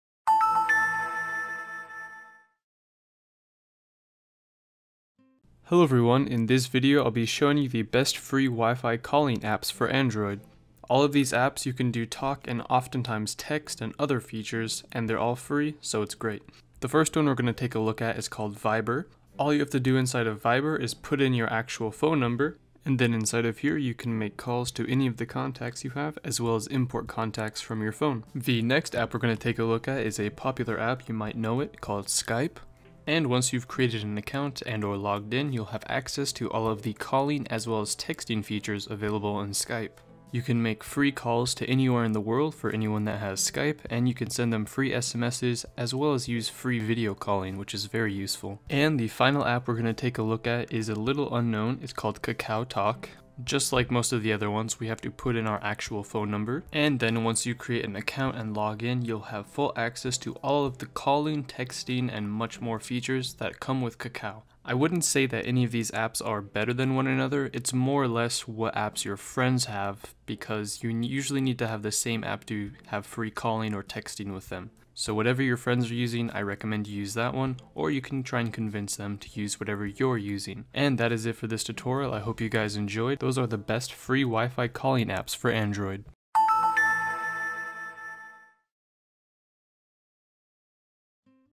Free Wi-Fi Calling App For Android Devices
Viber
It is the one of the most used free Wifi calling app for Android that helps to send free messages and calls on any network even through your tablet.
· The app easily synchronizes with the mobile contact list and detects automatically those contacts that have Viber.
· It integrates with Android OS to give the best performance as it allows you to send sticker, photo, video and voice messages along with phone calls and text.
· Viber has the “Hold & Talk” feature to send instant voice messages in real time.

Skype
· This free Wifi calling app for Android is considered as the most popular application for video calls, sending instant messages, SMS and mobile calls.
· It allows you to chat regardless of the service your friend is using.
· It is adaptable to any configuration, network speed and webcam.